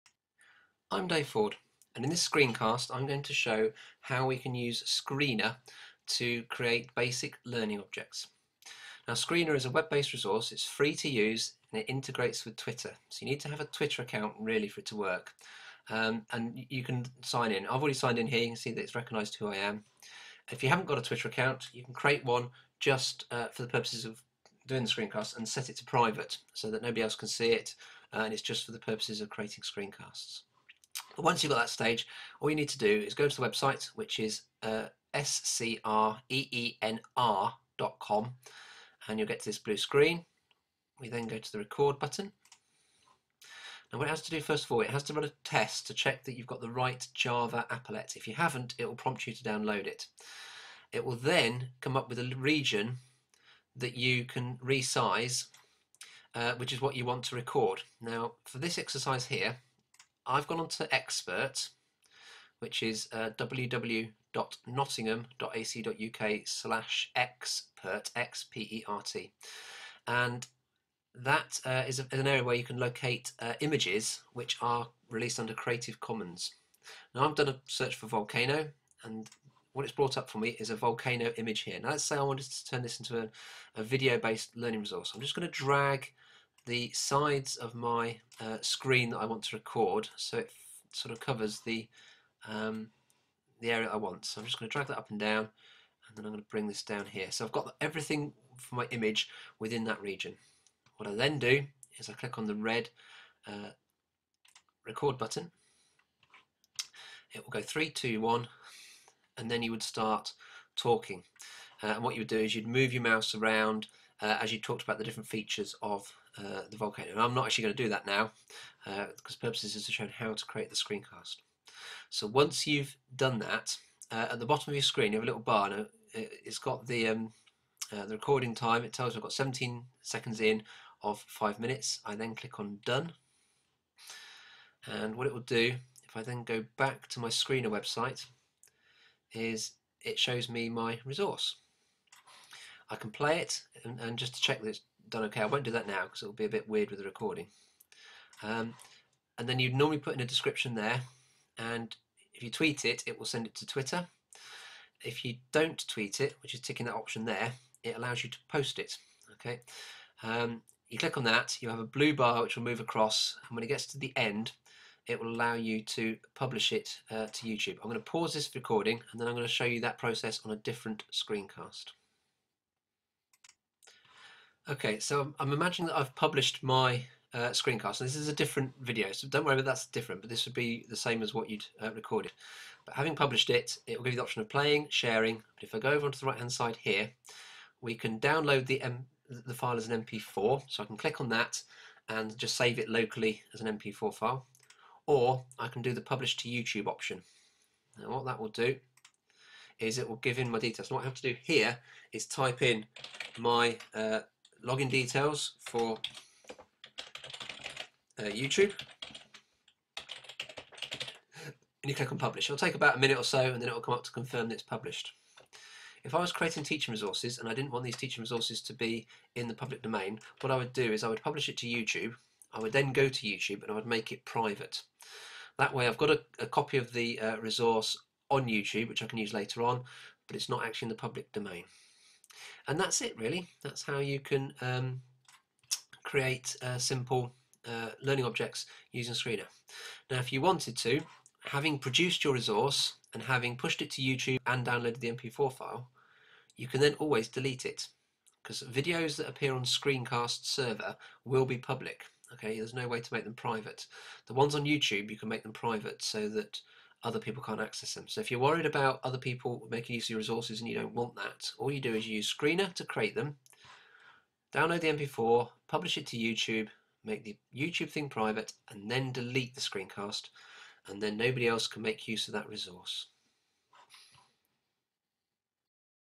How to use Screenr to make learning resources and keep them private
Screencast showing how to use screenr to make learning resources, and keep them private.